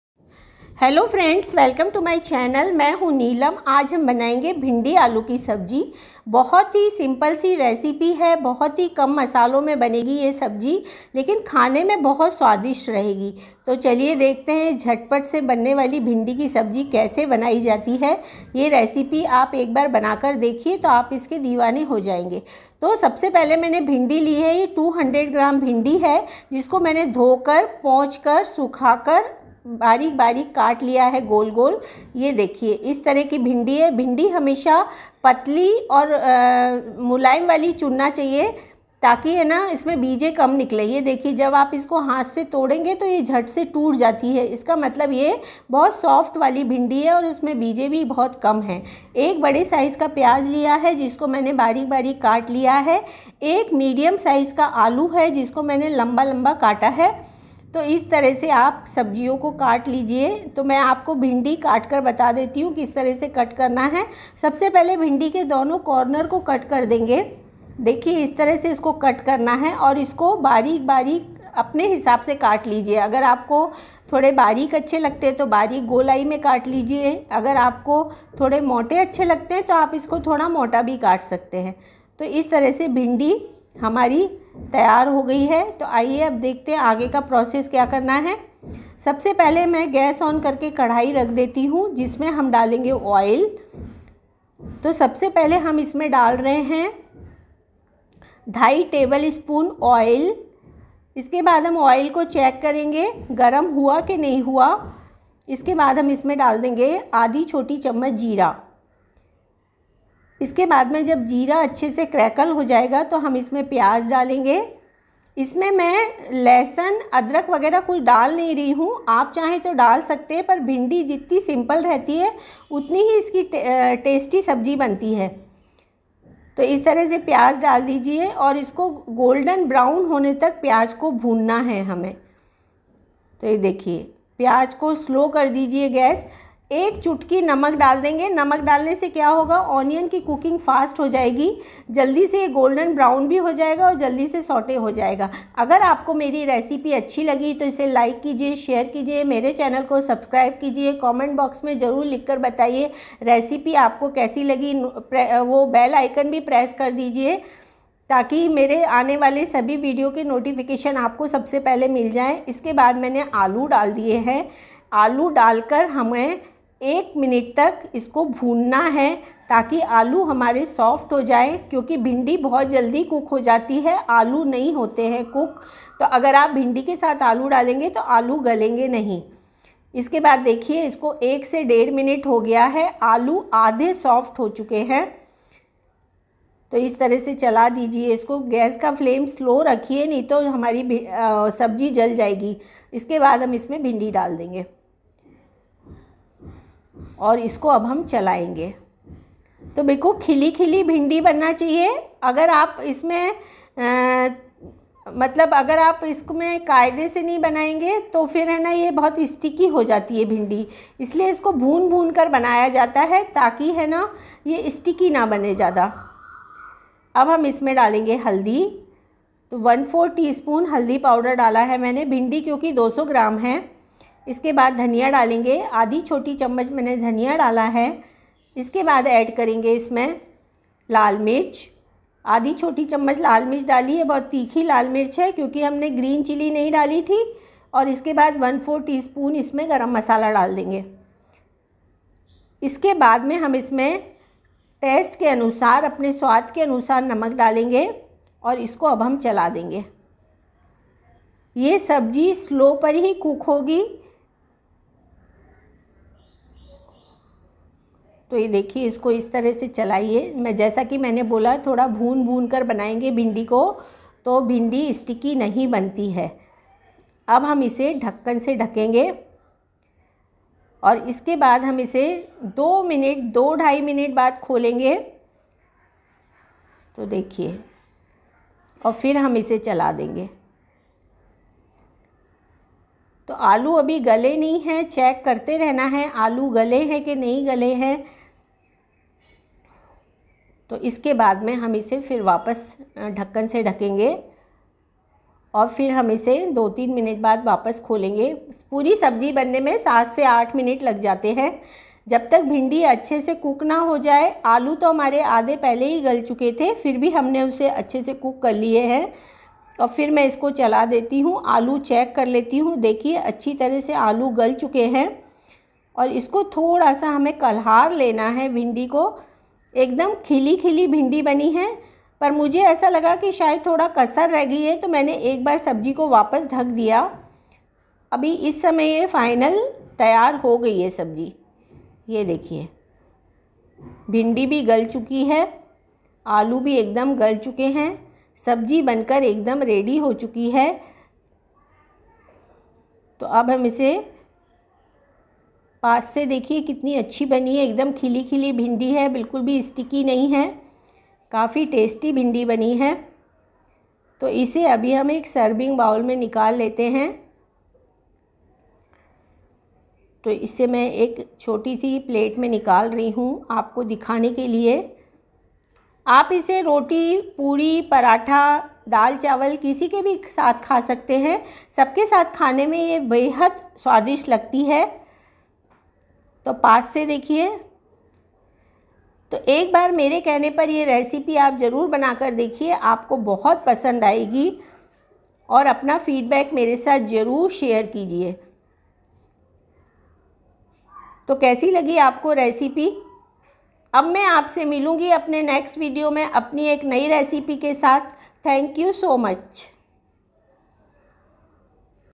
भिंडी आलू की मजेदार सब्जी | delicious bhindi aloo fry | ladyfinger potato sabji
Bhindi aloo ki sabji.. Iss recipe main maine bataya hai ki quick and easy tarike se bhindi aloo ki sukhi sabji kaise banaye ye sabji bahut hi swadisht hoti hai